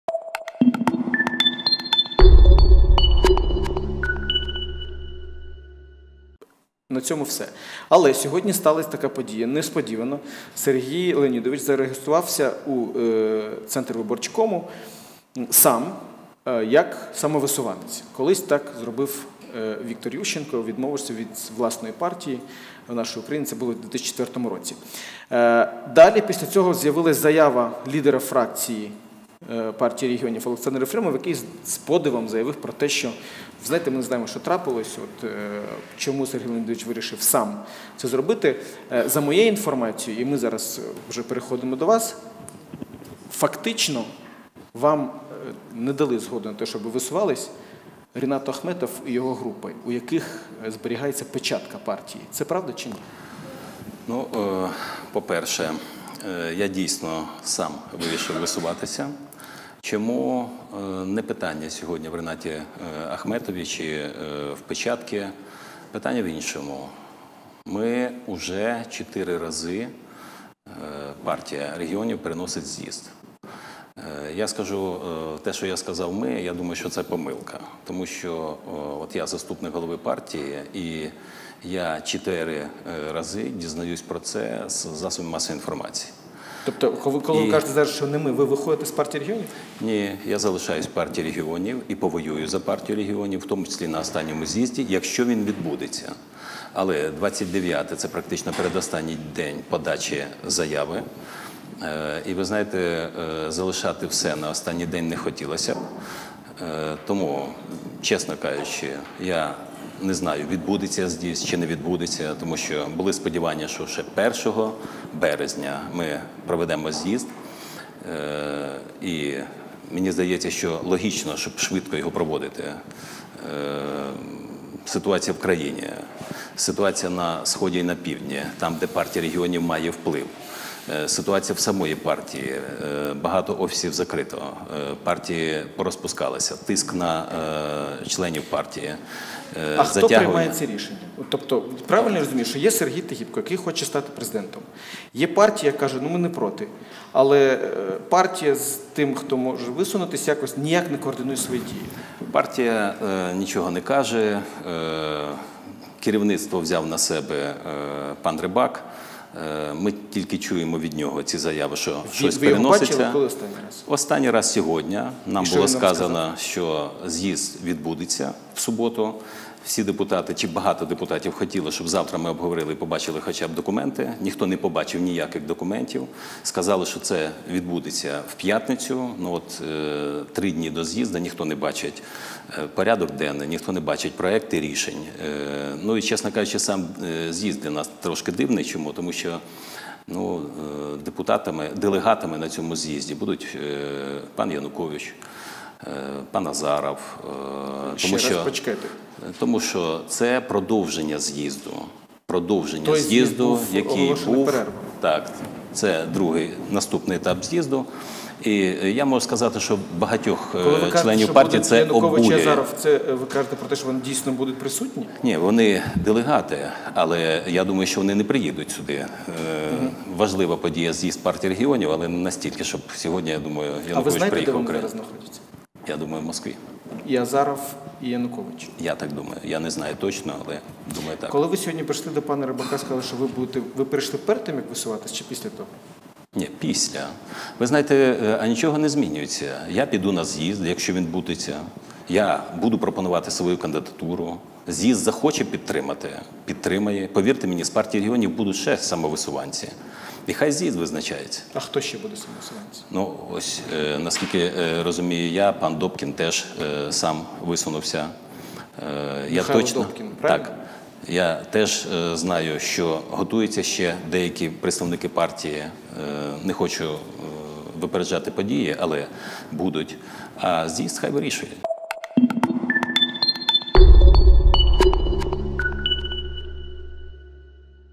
Сергій Тигіпко про участь у виборах Президента
Сергій Тигіпко в Студії Громадського 25.03.14